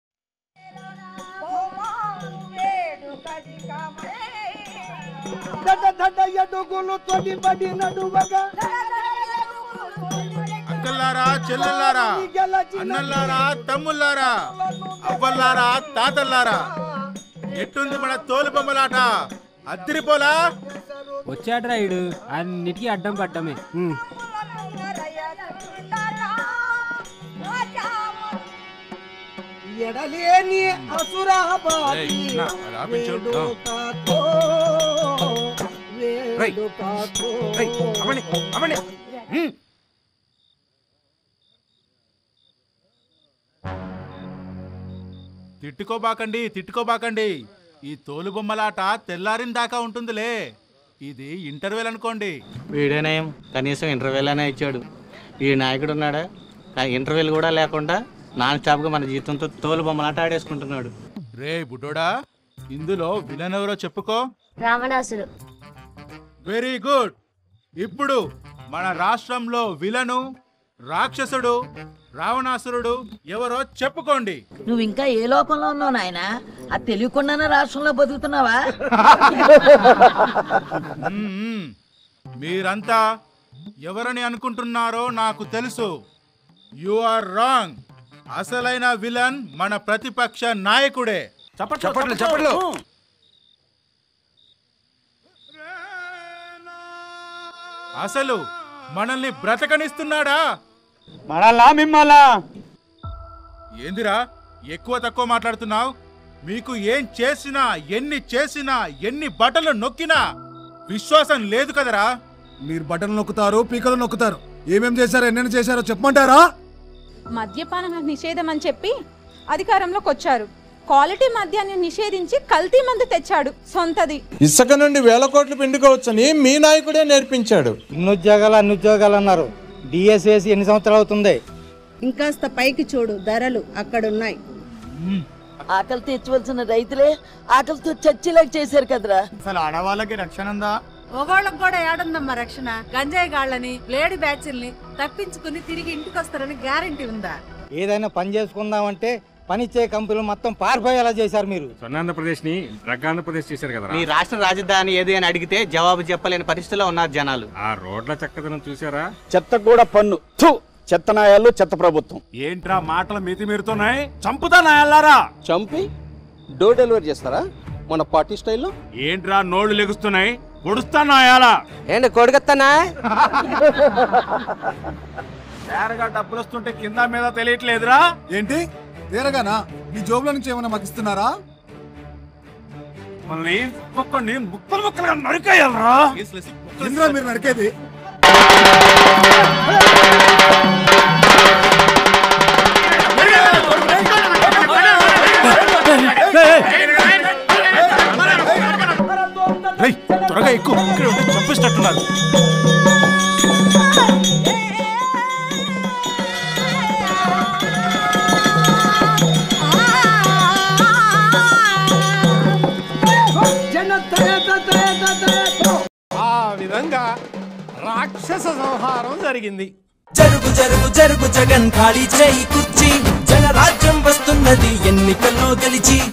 వైసీపీ రాక్షస సంహారంపై టీడీపీ అదిరిపోయే యాడ్ | TDP Short Films | YCP Government | YS Jagan
Watch Here : వైసీపీ రాక్షస సంహారంపై టీడీపీ అదిరిపోయే యాడ్ | TDP Short Films | YCP Government | YS Jagan